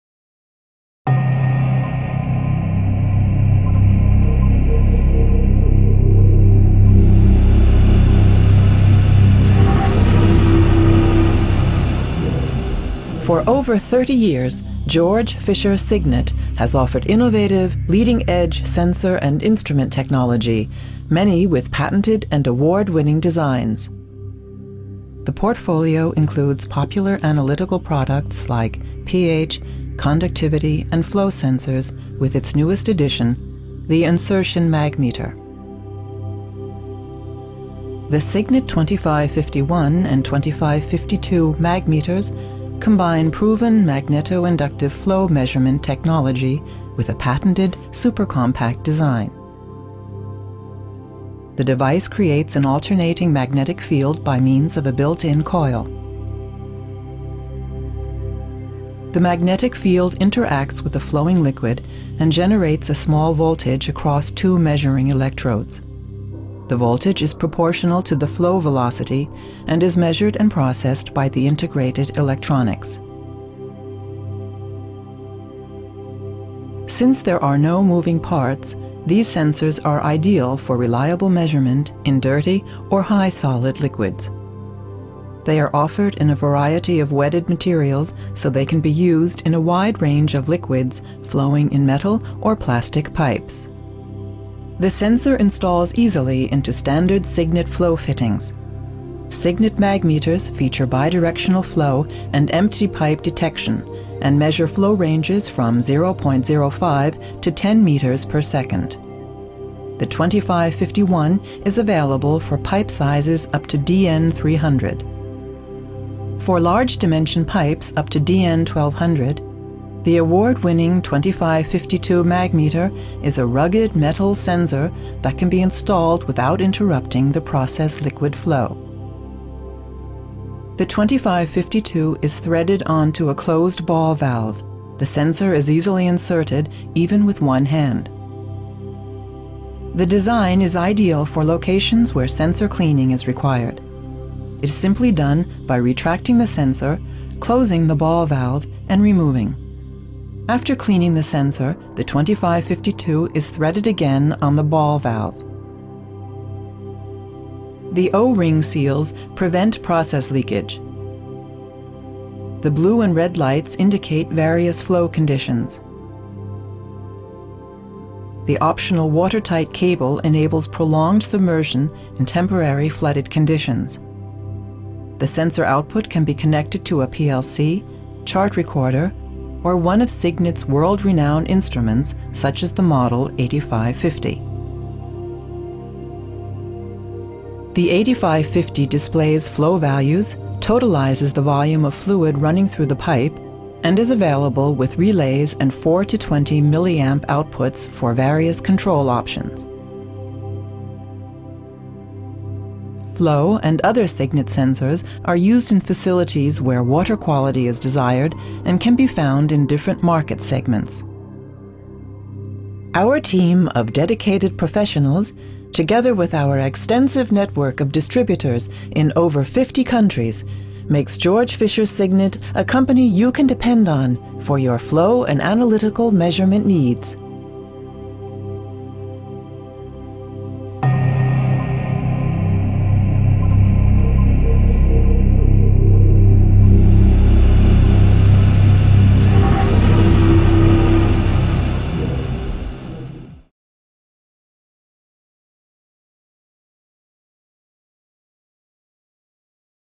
GF Signet 2551 Magmeter | Instrumart
The GF Signet 2551 Magmeter is an insertion-style magnetic flow sensor without any moving parts. All versions of the GF Signet 2551 Magmeters are constructed of corrosion-resistant materials to provide long-term reliability with minimal maintenance costs.